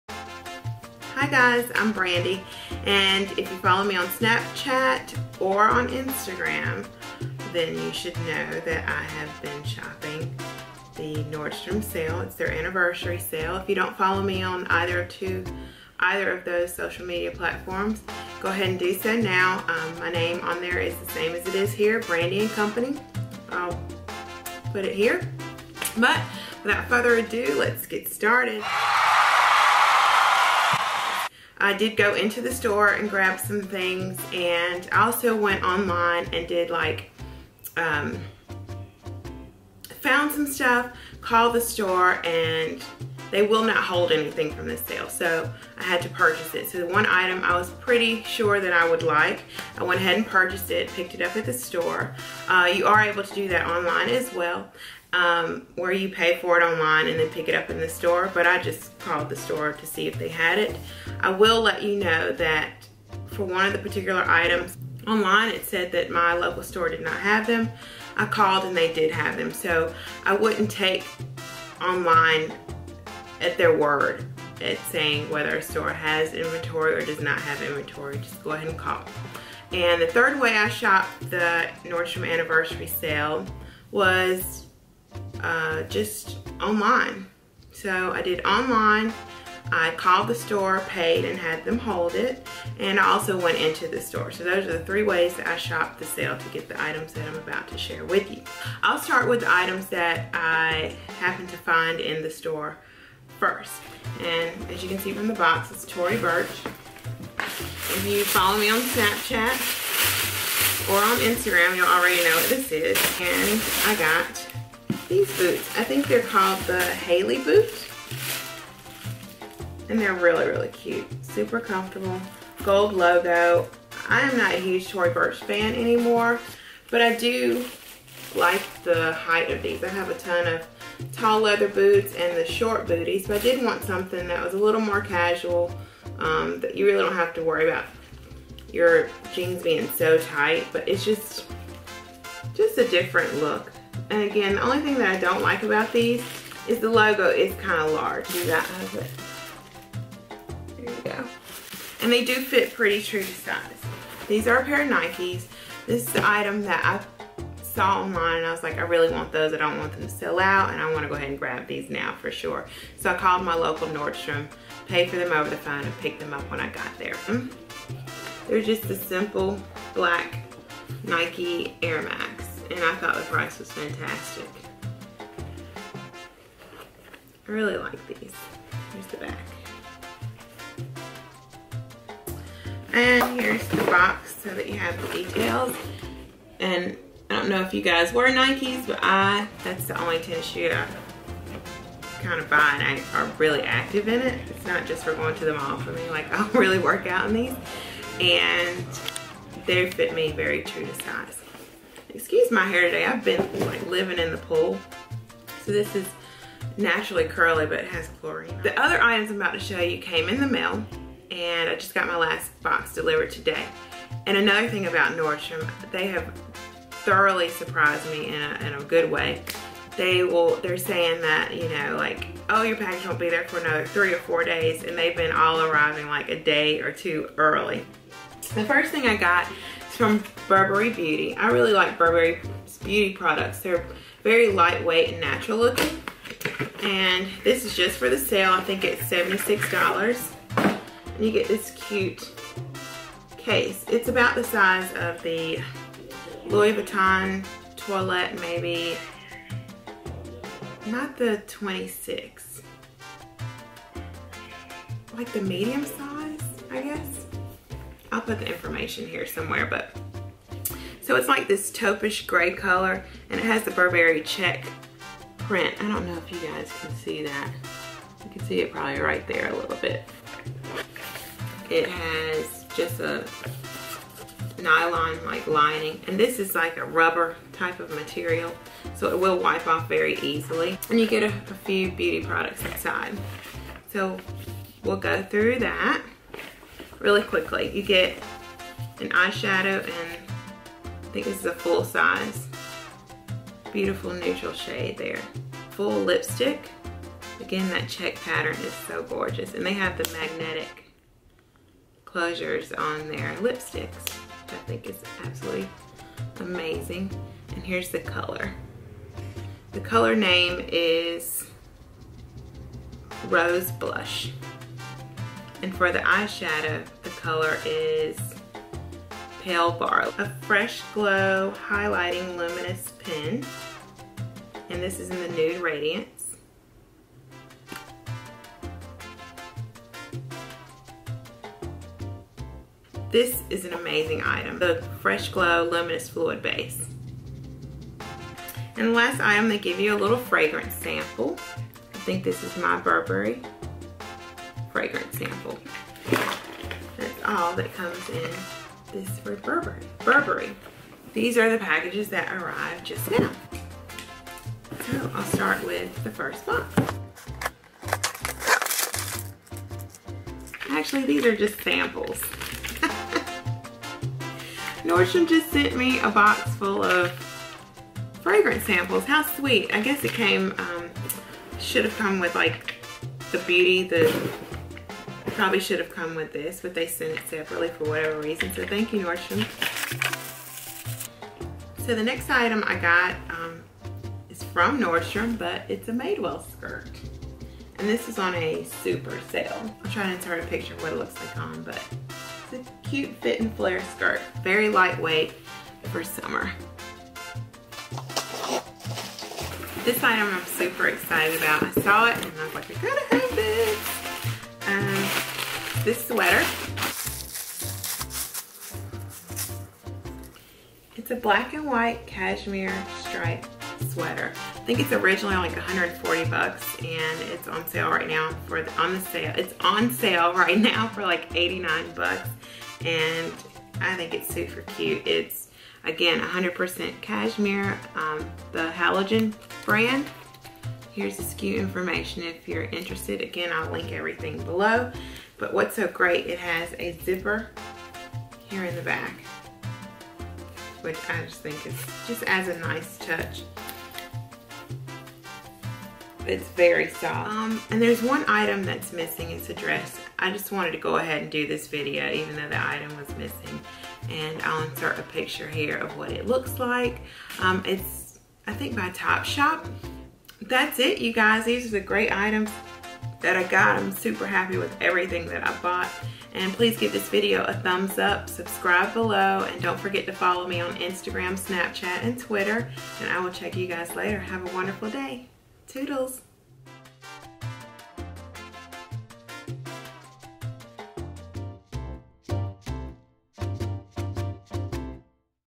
Nordstrom Anniversary Sale | Tory Burch | Nike | Burberry | TOPSHOP
I hope you enjoy my items found during the Nordstrom Anniversary Sale!  Please see below for all items featured in this video.